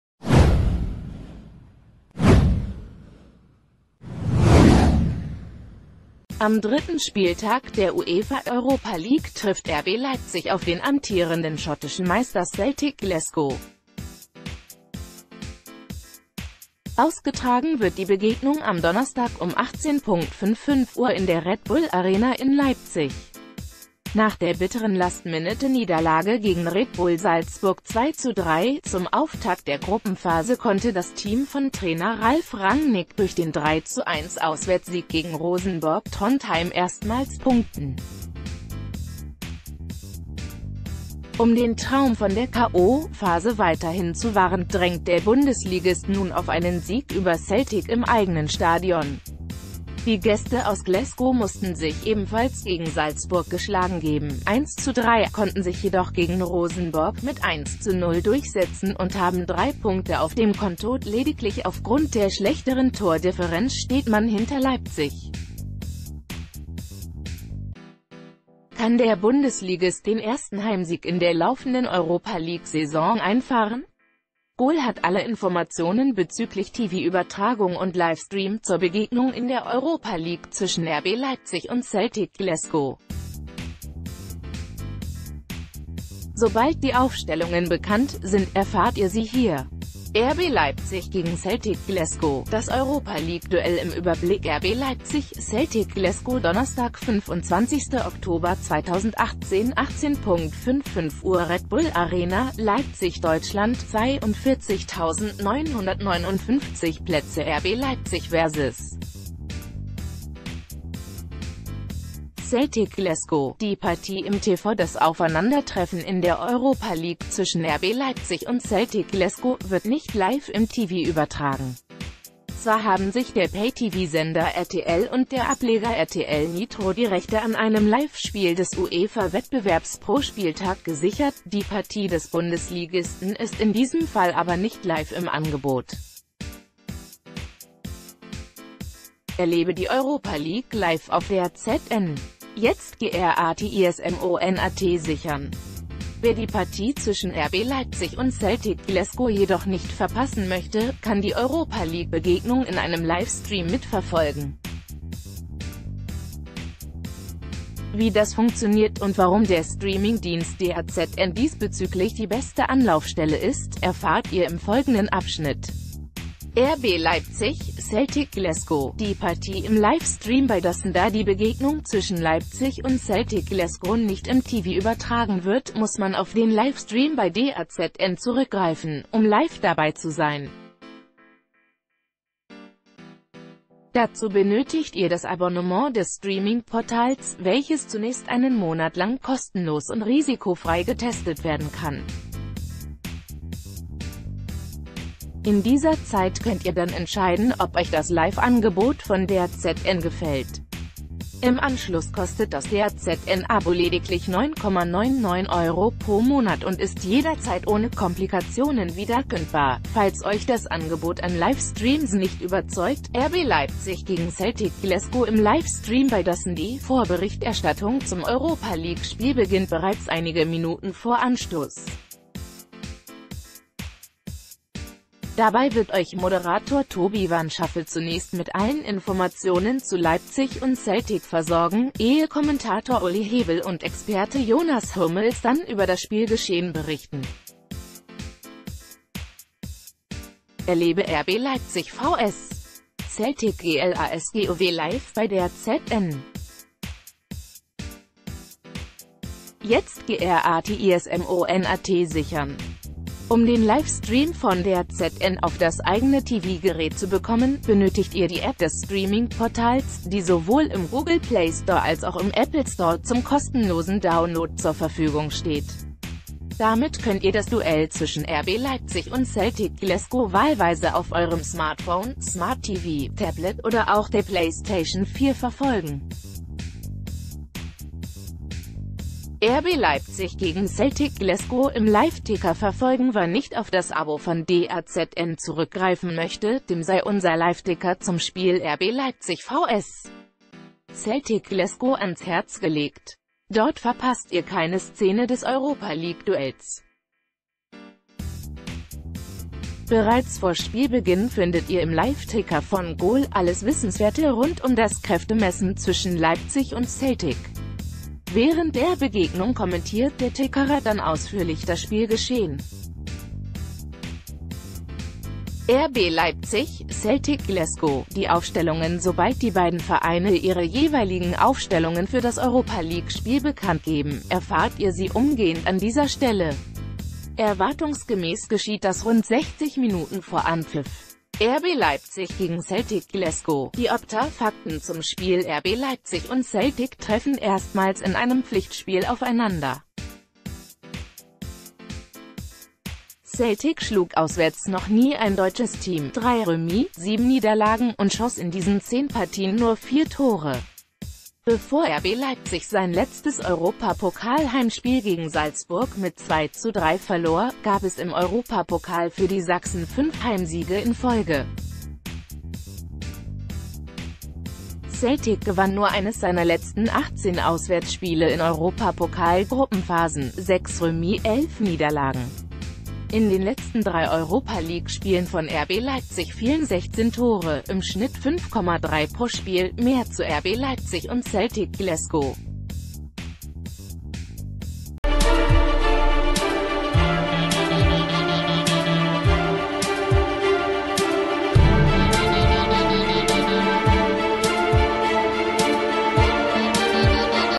Leipzig empfängt Celtic Glasgow: TV, LIVE-STREAM, Ticker
Am dritten Spieltag der UEFA Europa League trifft RB Leipzig  auf den amtierenden schottischen Meister Celtic Glasgow. Ausgetragen wird die Begegnung am Donnerstag um 18.55 Uhr in der Red Bull Arena in Leipzig. Nach der bitteren Last-Minute-Niederlage gegen Red Bull Salzburg (2:3) zum Auftakt der Gruppenphase konnte das Team von Trainer Ralf Rangnick durch den 3:1-Auswärtssieg gegen Rosenborg Trondheim erstmals punkten. Um den Traum von der K.o.-Phase weiterhin zu wahren, drängt der Bundesligist...